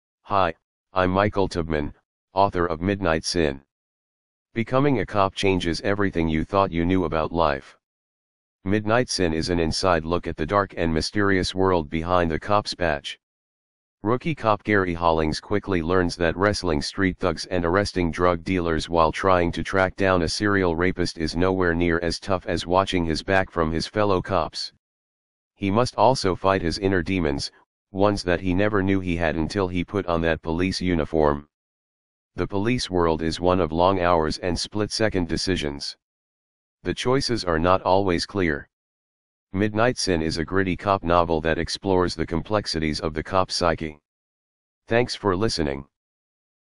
Michael Tabman Midnight Sin
Hi, I’m Michael Tabman, author of Midnight Sin. Becoming a cop changes everything you thought you knew about life.  Midnight Sin is an inside look at the dark and mysterious world behind the cop’s badge.  Rookie cop Gary Hollings quickly learns that wrestling street thugs and arresting drug dealers while trying to track down a serial rapist is nowhere near as tough as watching his back from his fellow cops.  He must also fight his inner demons – ones that he never knew he had until he put on that police uniform.
The police world is one of long hours and split-second decisions.  The choices are not always clear.  Midnight Sin is a gritty cop novel that explores the complexities of the cop psyche.  Thanks for listening!